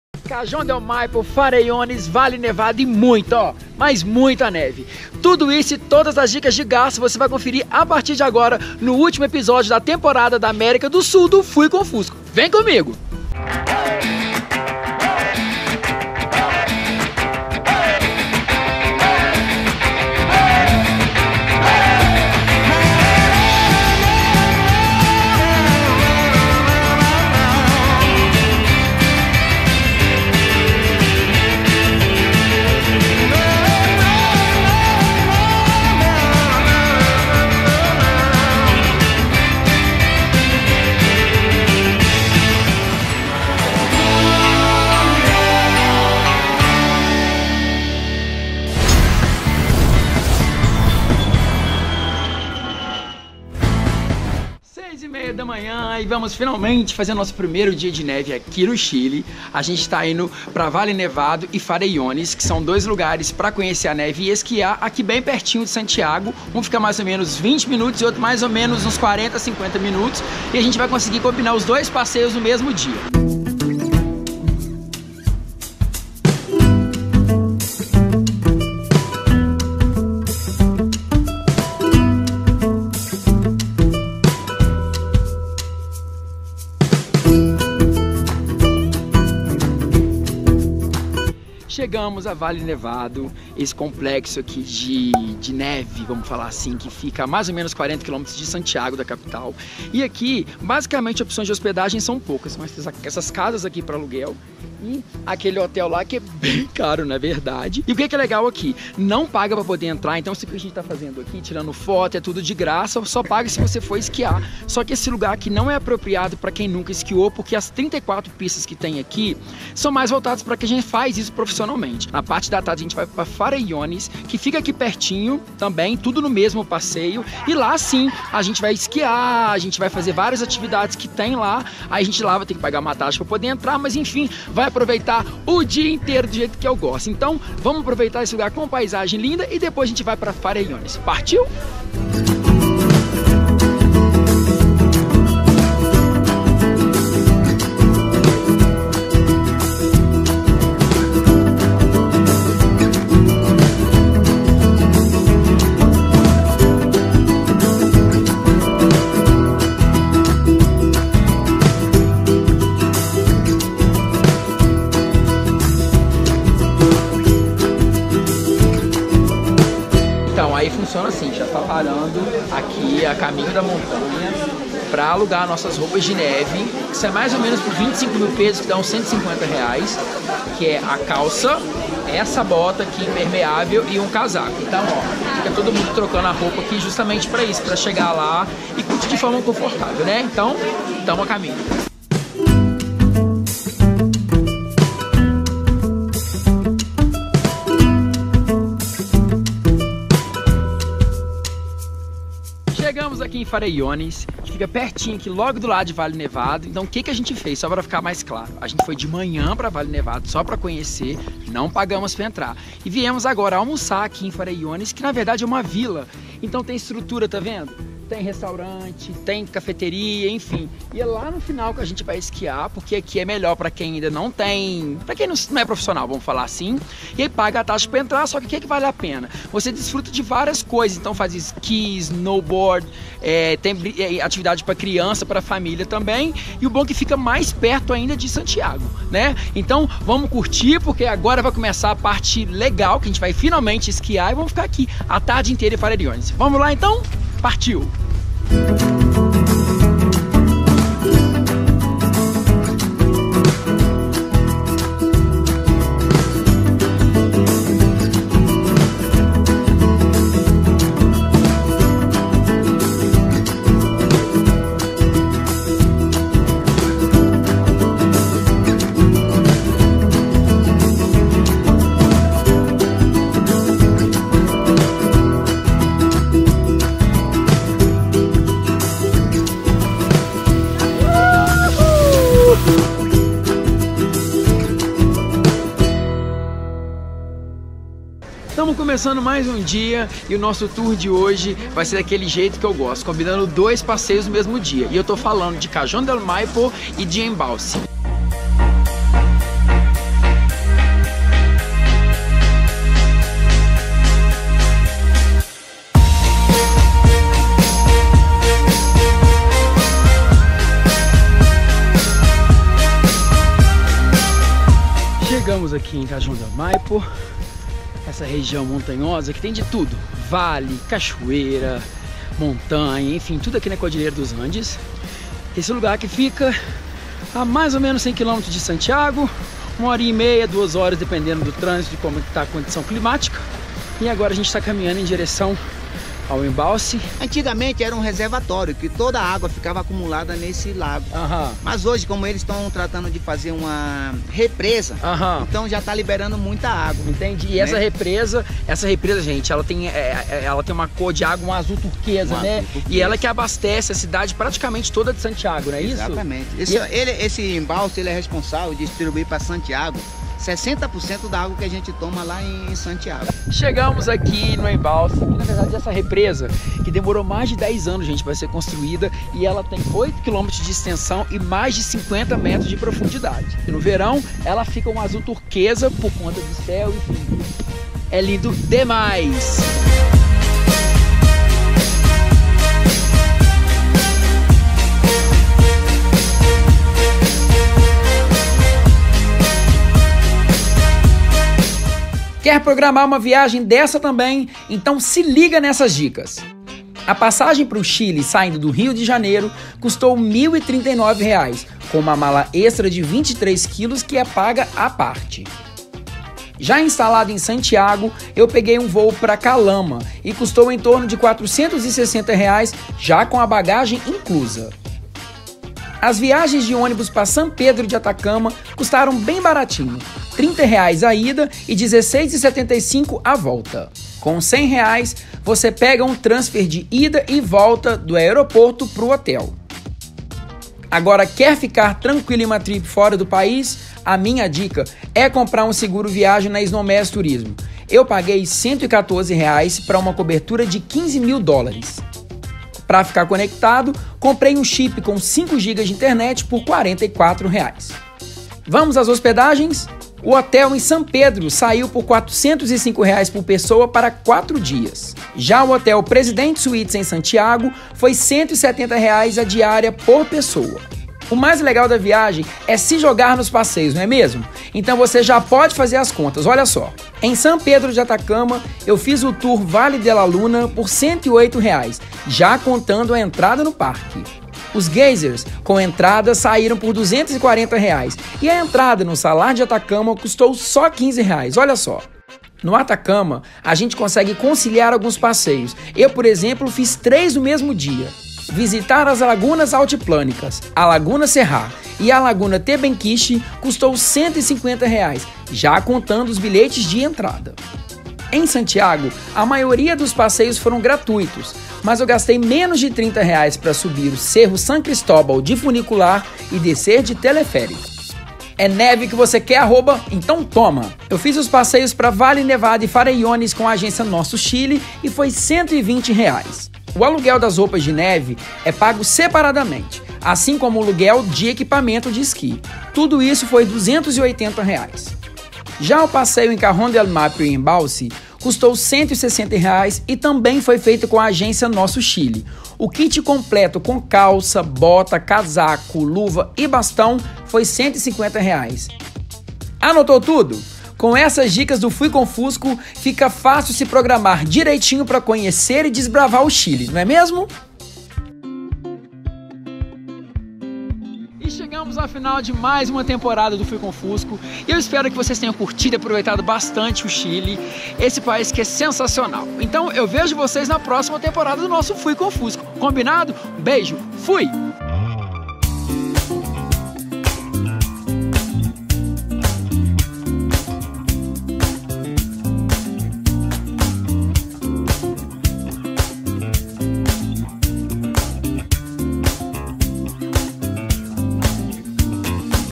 Fui com Fusco | Quanto custa esquiar e conhecer a neve no Chile
É neve que você quer @@? Então chega mais, que chegou a hora de pegar todas as dicas de como esquiar no Chile e economizando. Quer saber todos os detalhes de como realizar seu sonho de conhecer a neve? Chega mais que vou te mostrar tudo neste programa.

E aí, curtiu este programa? E se quiser curtir mais do Fusquinho só me acompanhar no Instagram @flaviofusco.